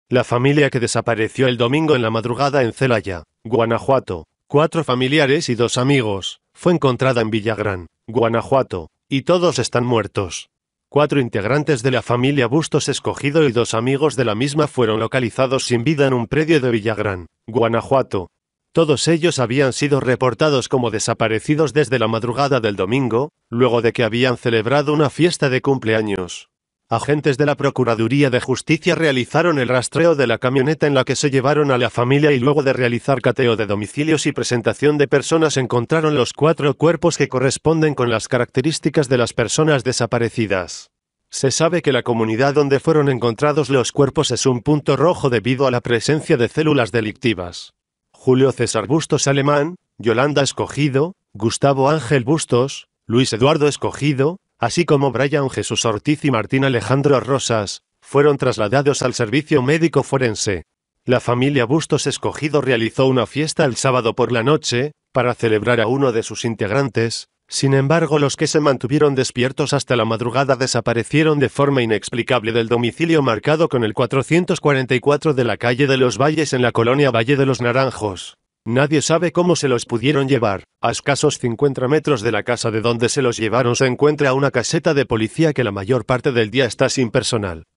Hallan muertos a integrantes de familia secuestrada en Celaya Video
La familia que desapareció el domingo en la madrugada en Celaya, Guanajuato, cuatro familiares y dos amigos, fue encontrada en Villagrán, Guanajuato, y todos están muertos.

Cuatro integrantes de la familia Bustos Escogido y dos amigos de la misma fueron localizados sin vida en un predio de Villagrán, Guanajuato.

Todos ellos habían Sido reportados como desaparecidos desde la madrugada del domingo, luego de que habían celebrado una fiesta de cumpleaños.

Agentes de la Procuraduría de Justicia realizaron el rastreo de la camioneta en la que se llevaron a la familia y luego de realizar cateo de domicilios y presentación de personas encontraron los cuatro cuerpos que corresponden con las características de las personas desaparecidas.

Se sabe que la comunidad donde fueron encontrados los cuerpos es un punto rojo debido a la presencia de células delictivas.

Julio César Bustos Alemán, Yolanda Escogido, Gustavo Ángel Bustos, Luis Eduardo Escogido, así como Brian Jesús Ortiz y Martín Alejandro Rosas, fueron trasladados al Servicio Médico Forense.

La familia Bustos Escogido realizó una fiesta el sábado por la noche, para celebrar a uno de sus integrantes, sin embargo los que se mantuvieron despiertos hasta la madrugada desaparecieron de forma inexplicable del domicilio marcado con el 444 de la calle de Los Valles en la colonia Valle de los Naranjos.

Nadie sabe cómo se los pudieron llevar.  A escasos 50 metros de la casa de donde se los llevaron se encuentra una caseta de policía que la mayor parte del día está sin personal.